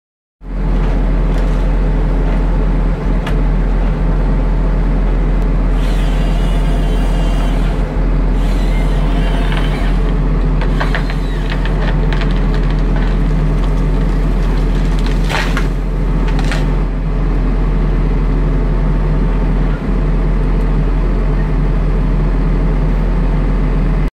Fällning av grov gran
En näst intill övergrov gran fälls med storskördaren som inte riktigt når runt stammen.